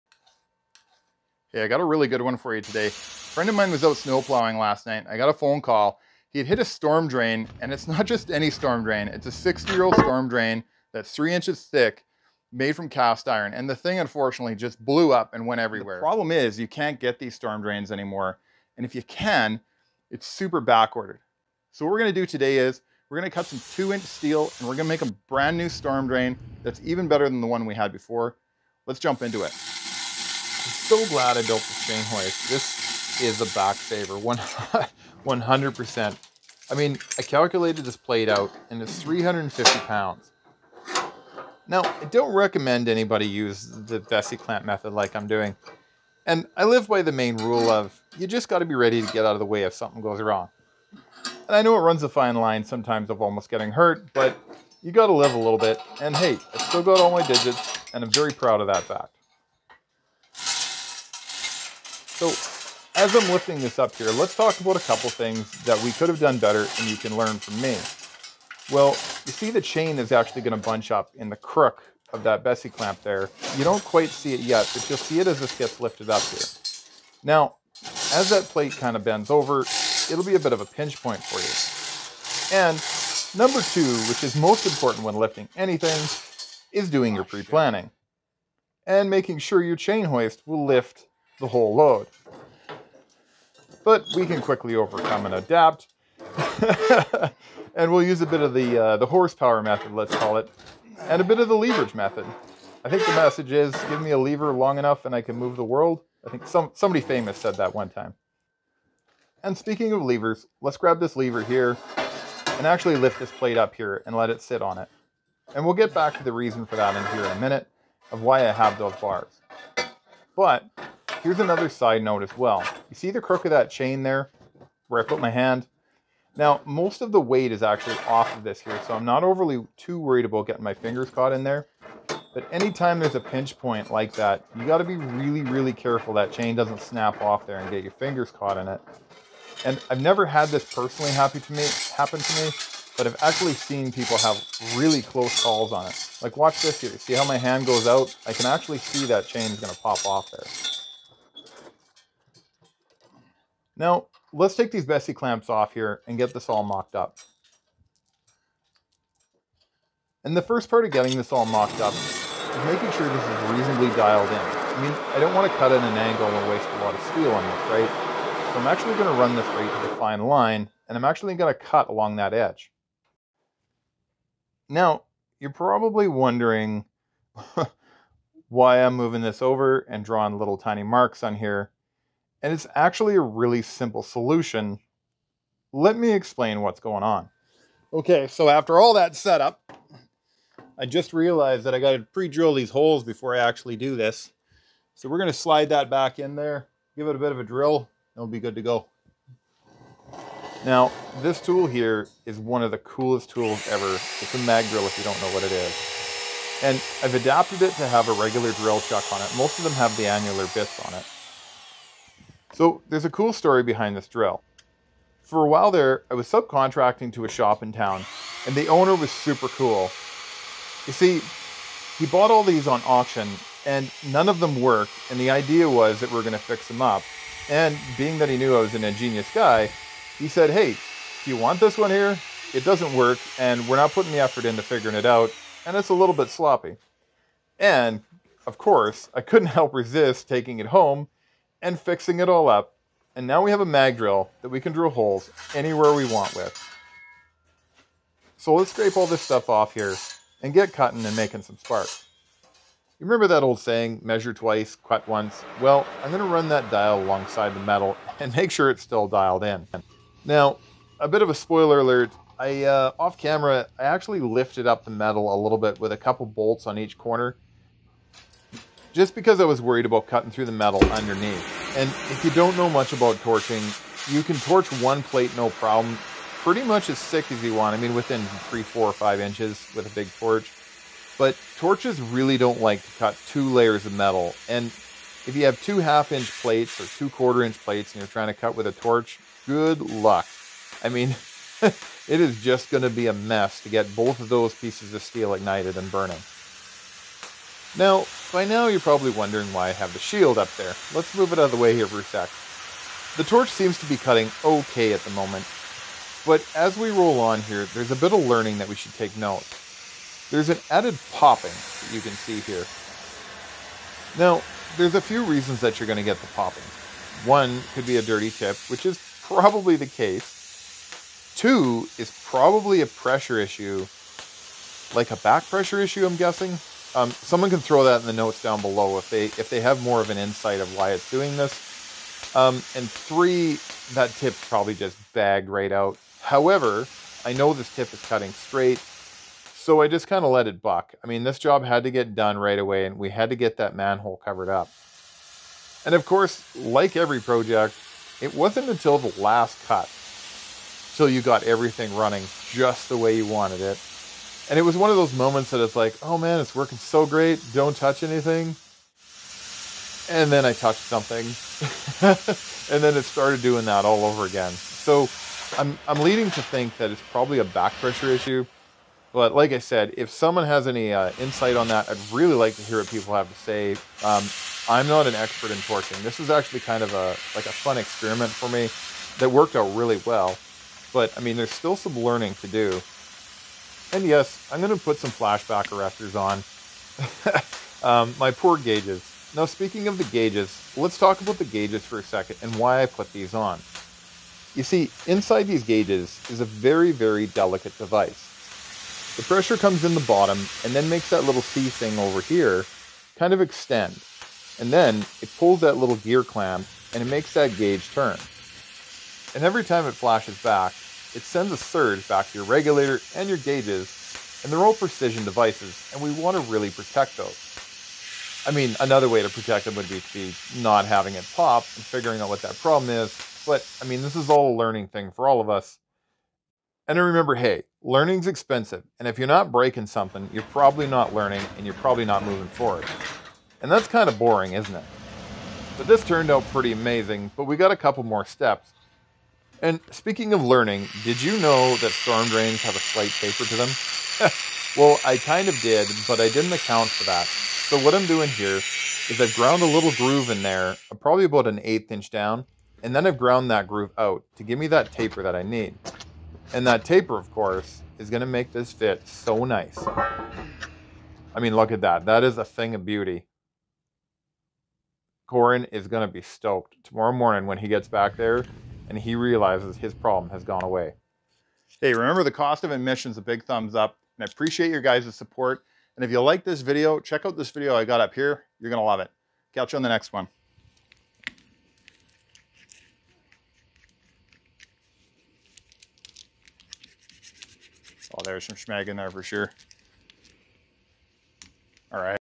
Cutting thick steel plate in a small job shop
Taking measurements from ancient broken storm drain we take a massive 2" steel plate and torch cut and manufacture a brand new one. Then we take it out at night and throw it in place and call the contractor and let him know he can rest easy that the job is done .